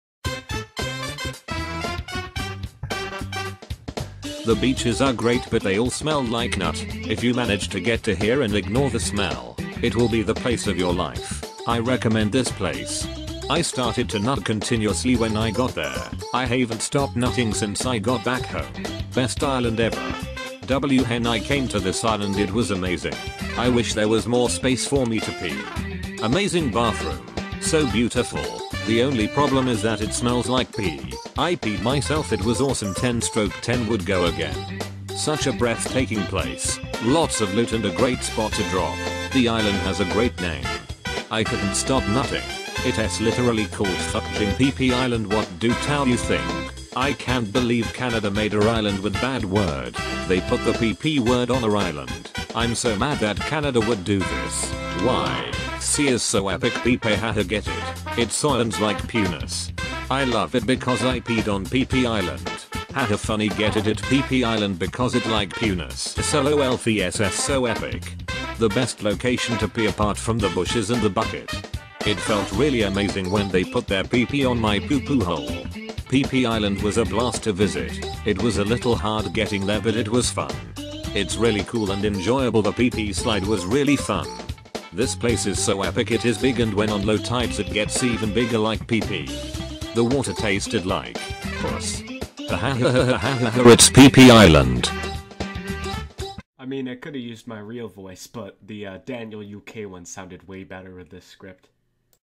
reading Google Maps reviews for Pee Pee island
HAHAHHAAHHAHAHAHAHA ITS PEE PEE ISLAND
(Canadians are weird)

Want to collab or talk with me? heres my EPICaL DISCORD